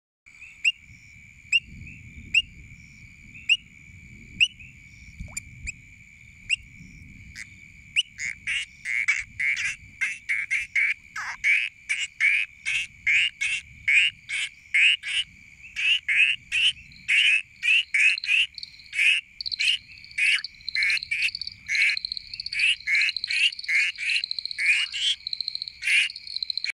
Cute frog makes ALIEN sound #3
Two painted reed frogs calling a potential mate
Hyperolius marmoratus